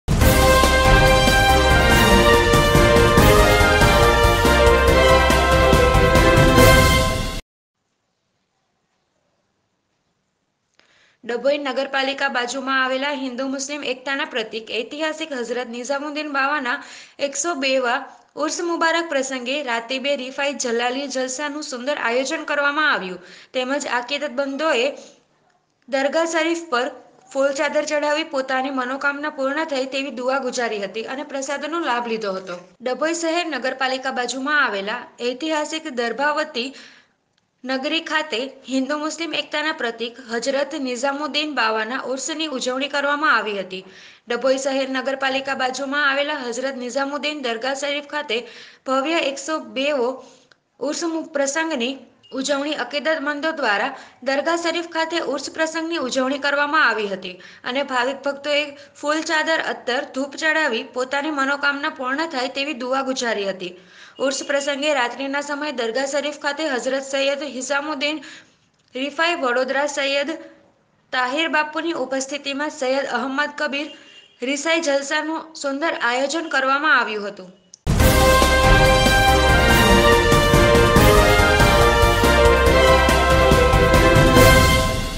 ડભોઇ નગરપાલિકા બાજુમાં આવેલા હિન્દુ મુસ્લિમ એકતાના પ્રતીક ઐતિહાસિક હઝરત નિઝામુદ્દીન
ડભોઇ નગરપાલિકા બાજુમાં   આવેલા હિન્દુ મુસ્લિમ એકતાના પ્રતીક ઐતિહાસિક હઝરત નિઝામુદ્દીન બાવા નો ઉસૅ નુ સુંદર આયોજન કરવામાં આવ્યું હતું